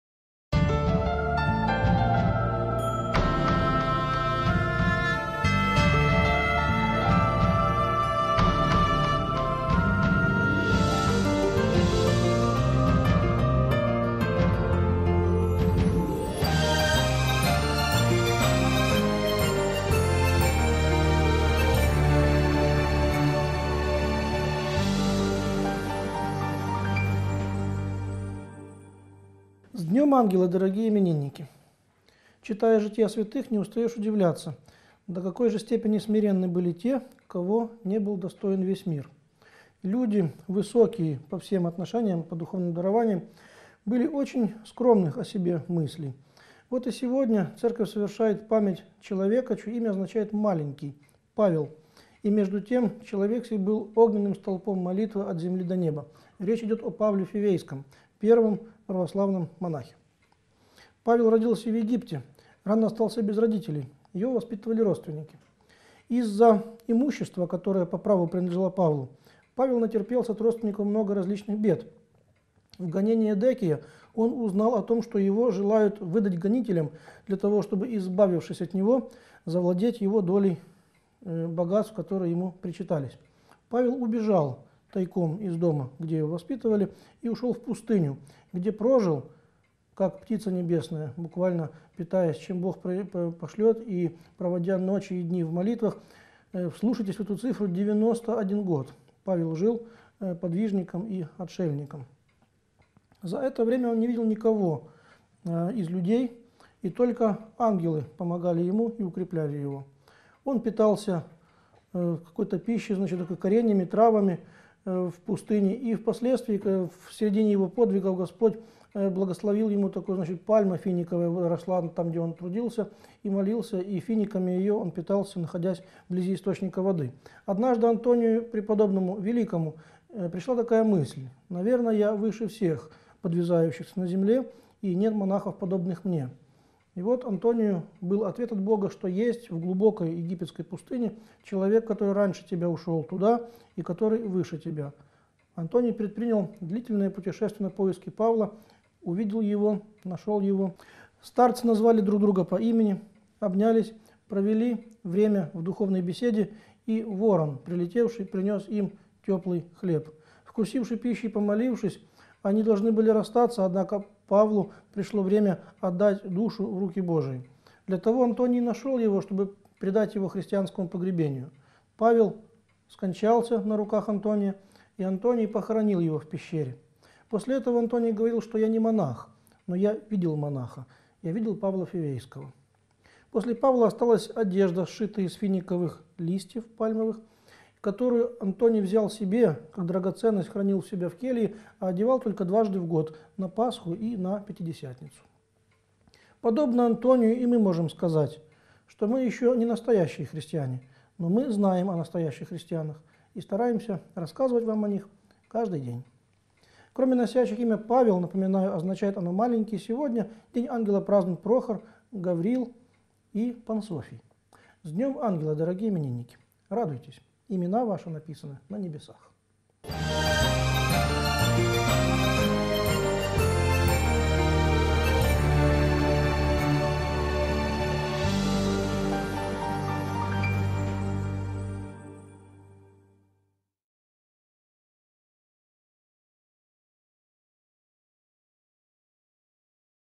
День ангела 28 января – День преподобного Павла Фивейского – отец Андрей Ткачёв
28 января именины празднуют Алёна, Елена, Варлаам, Гавриил, Герасим, Иван, Михаил, Павел, Прохор, Ян. В этот день церковь почитает память преподобного Павла Фивейского.

Распространи это видео!